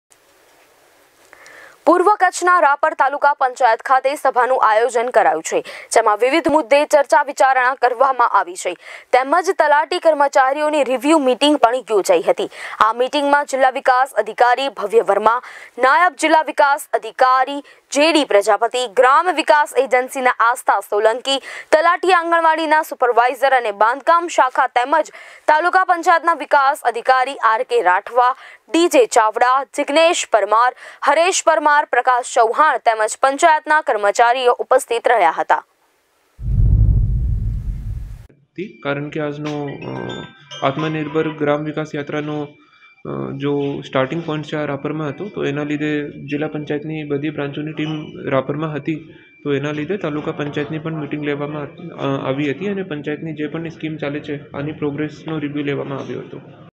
News | રાપર તાલુકા પંચાયત મધ્યે મીટીંગ સભા હોલ ખાતે યોજાઇ | VR LIVE
Reporting By હીરાલાલ વાઘેલા

Anchor - ધ્વની,સોનલ,દિપાલી,ક્રિશ્ના |  Copy Editor - વરુણ